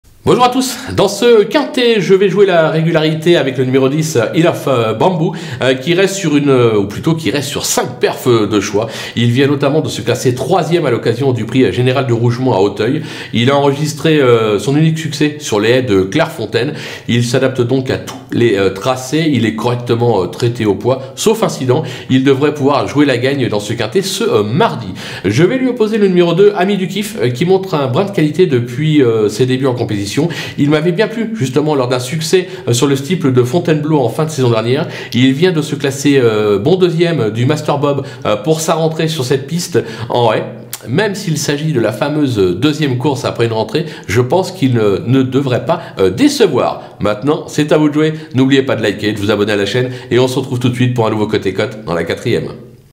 COTE & COTE - PRONOSTICS QUINTÉ PRIX GASTON BRANÈRE | COMPIÈGNE (R1-C3) | 25/04/2023
Des bases en bétons, des outsiders séduisants, des coups de poker osés, des tocards repérés dans le quinté du jour…Suivre les pronos de Philippe Thévenon, c’est l’assurance d’avoir toujours la Cote & Cote !